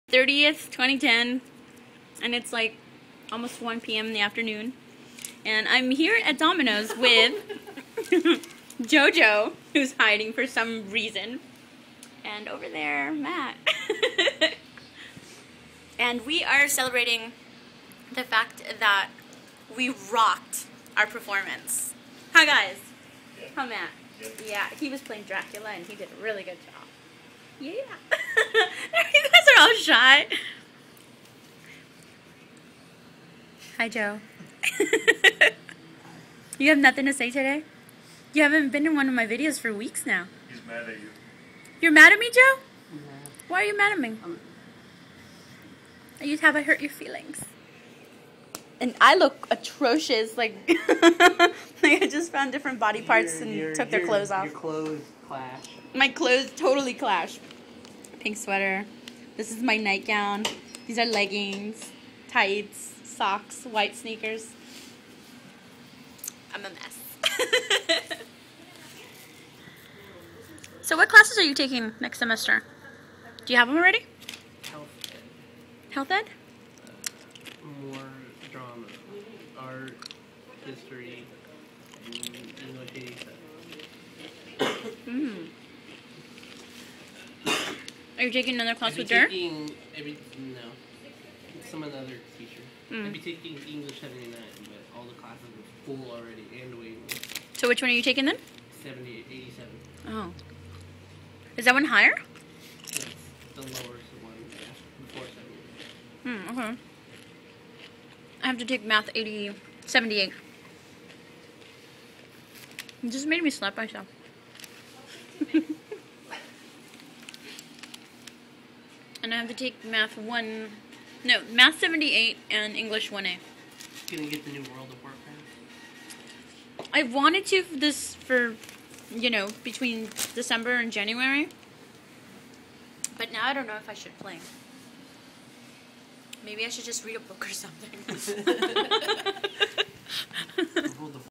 Jojo Matt n Kat
Going to Azi's house to hangout, goofin around while we wait for pizza.